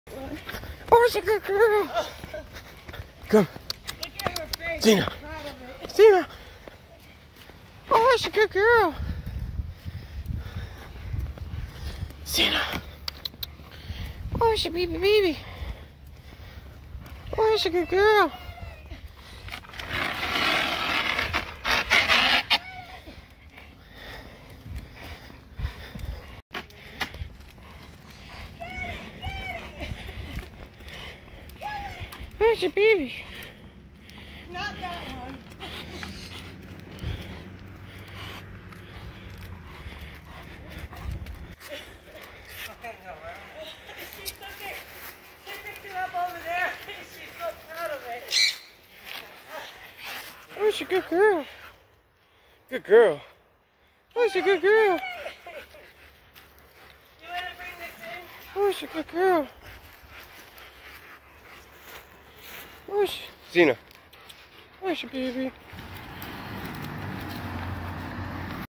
Dog steals a stick from someone's Yard
Dog snags a stick from someone's yard. She's so proud of it wants to bring it home.